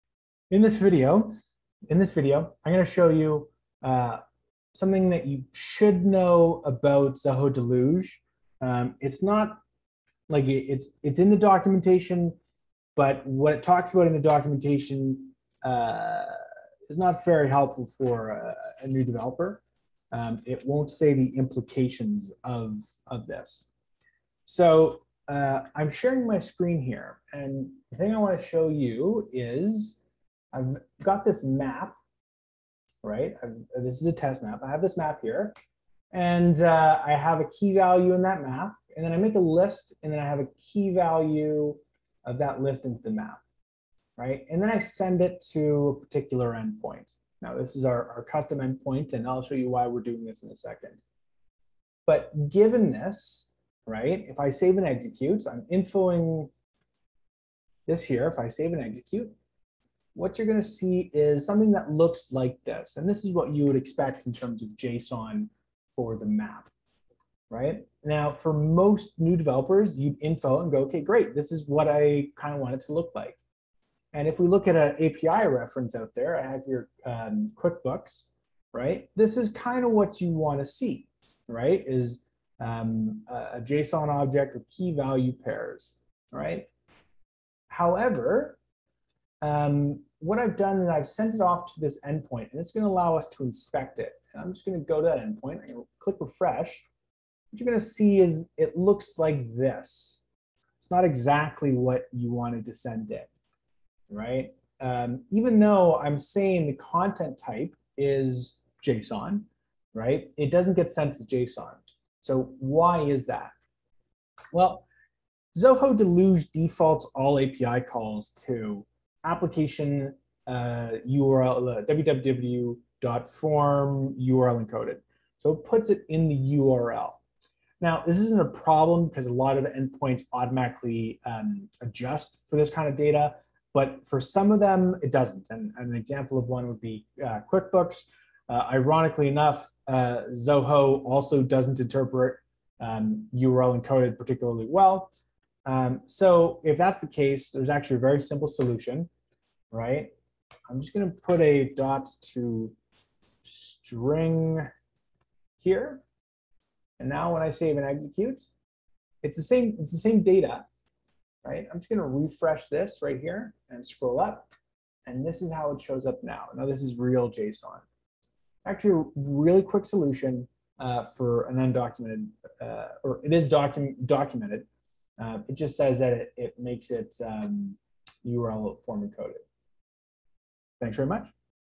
Zoho CRM Deluge Webhooks JSON Issue
If you're new to Deluge, Zoho CRM, and API integrations, this may be something you're about to encounter. The Webhook Body out of Deluge automatically changes to URLEncoded. Heres the workaround for this issue

Chapters:
00:00 Introduction
00:23 Bad Code
2:27 Very Simple Solution